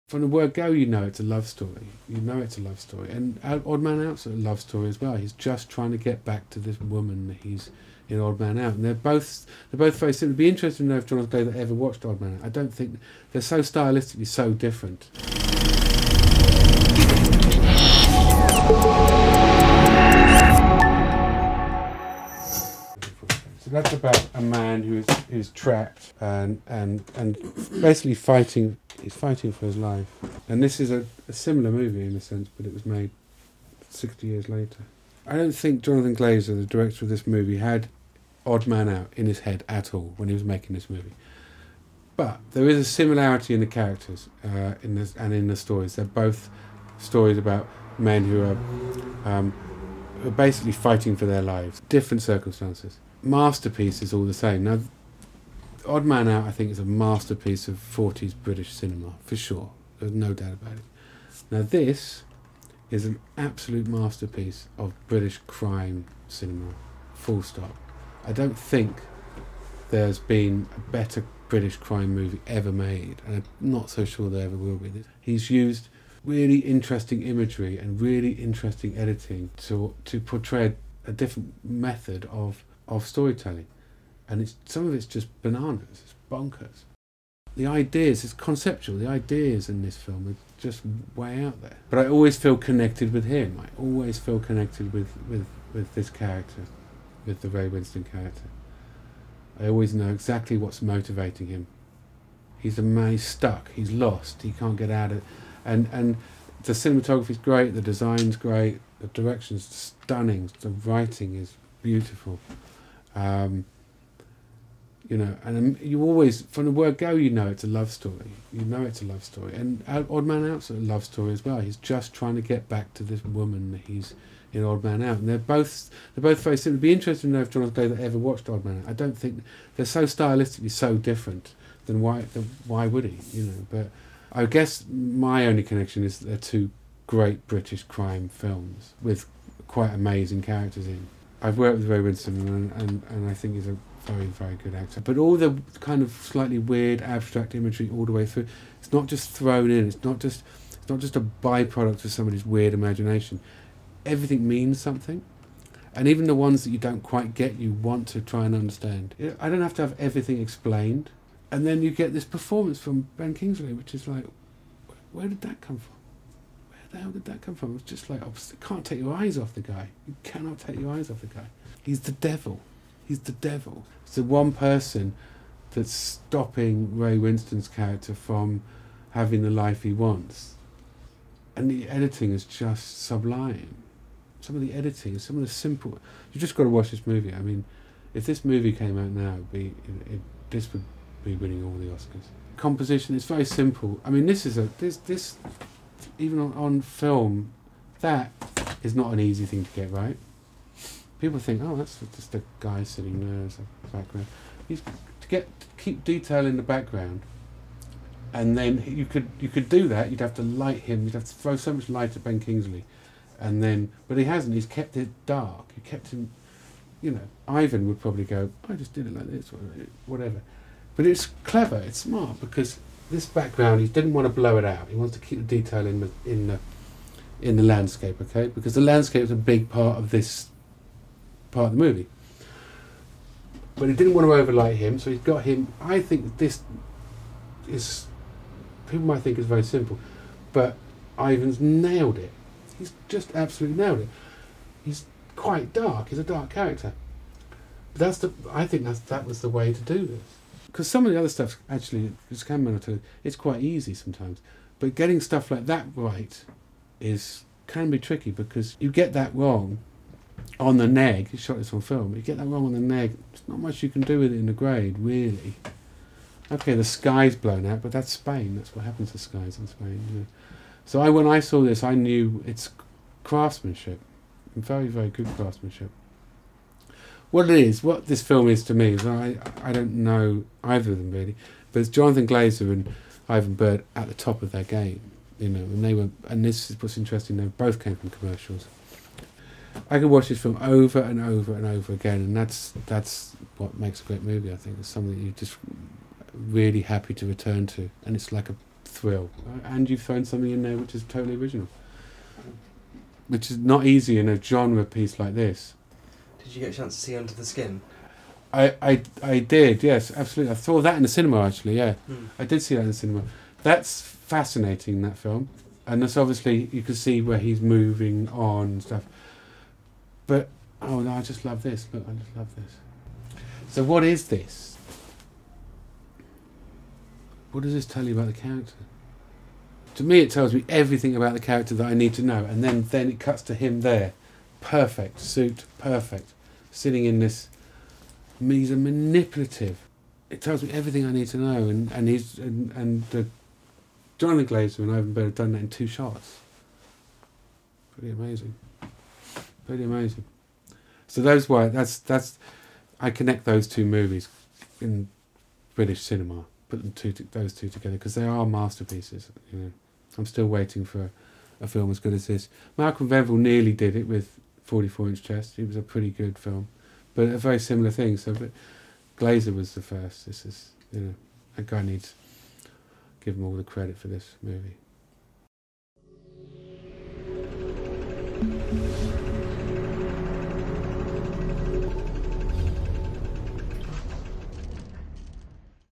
Sexy Beast Cinematography Analysis (Part 2) || Deconstructing Cinematography || Ben Smithard
Here is another Deconstructing Cinematography episode in which Ben Smithard analyses the cult classic Sexy beast.